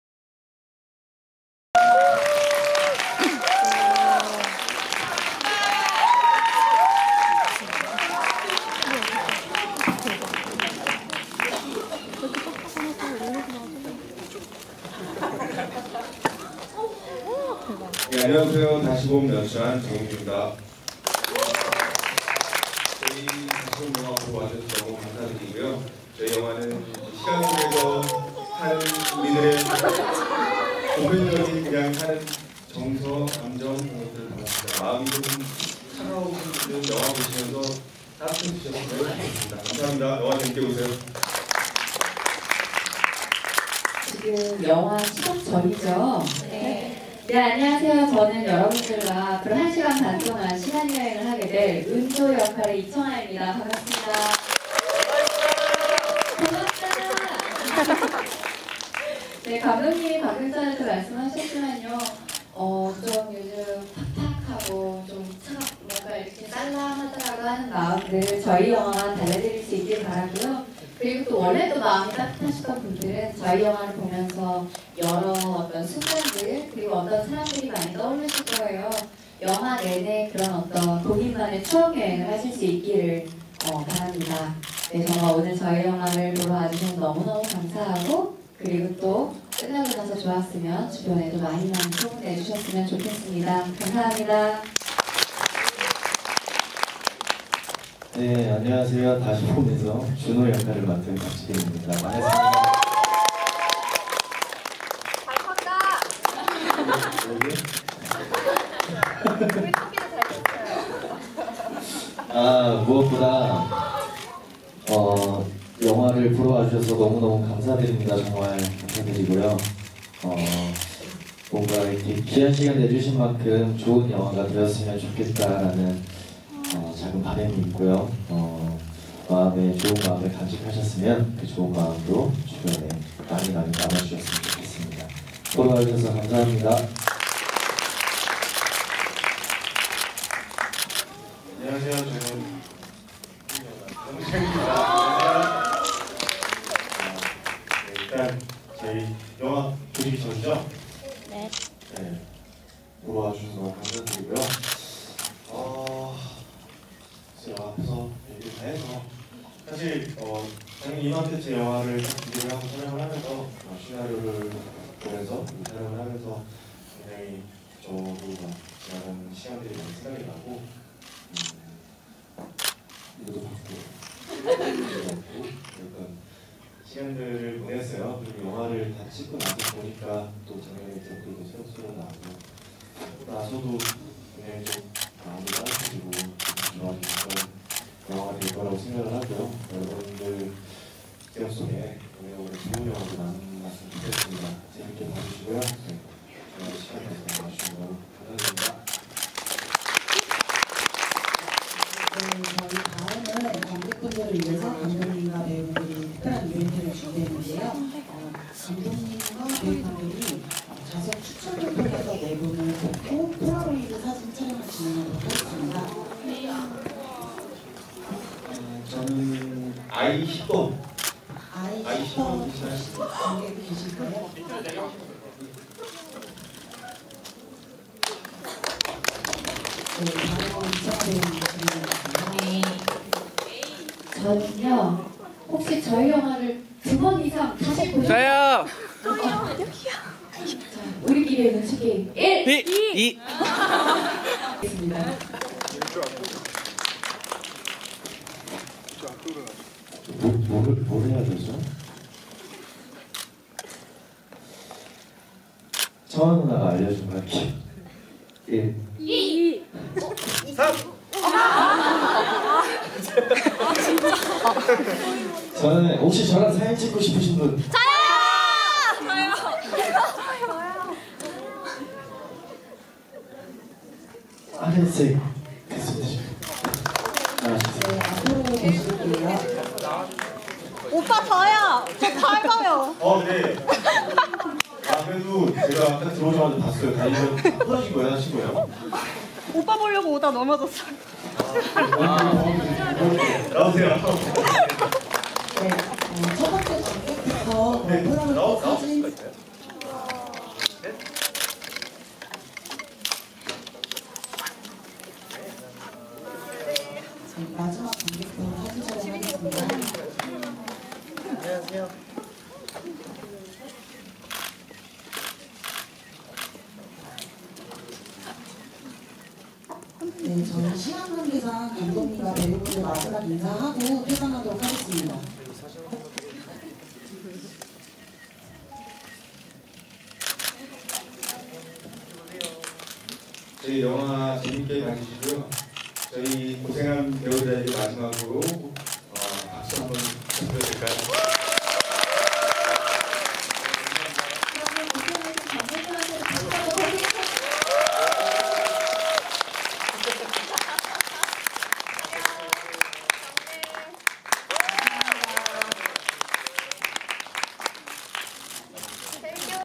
190421 색다른 타임슬립 영화 이청아X홍종현X박지빈 무대인사, 롯데시네마 월드타워 '다시 봄'
21일 오후 서울시 송파구 신청동 롯데시네마 월드타워에서 영화 '다시 봄' 무대인사 행사가 열려 배우 이청아, 박지빈, 홍종현과 정용주 감독이 참석해 관객과 소통을 하고 있다.

한편 '다시 봄'은 인생의 유일한 행복인 딸을 사고로 잃은 후 절망에 빠진 '은조' 중대한 결심을 그날, 어제로 하루씩 거꾸로 흘러가는 시간을 살게되면서 인생 두번째 기회를 얻게 된 타임 리와인드 무비로 현재 절찬 상영중이다.